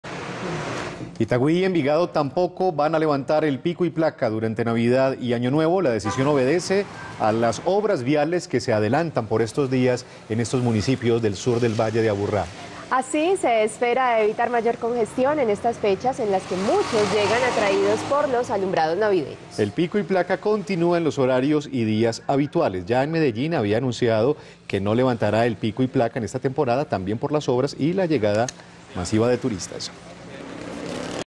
Itagüí y Envigado tampoco levantarán el pico durante navidad y año nuevo
La decisión obedece a las obras viales que se adelantan por estos días en estos municipios del sur del Valle de Aburrá.

Así se espera evitar mayor congestión en estas fechas en las que muchos llegan atraídos por los alumbrados navideños. El pico y placa continúa en los horarios y días habituales.

Ya Medellín había anunciado que no levantará el pico y placa en esta temporada, también por las obras y la llegada de turistas.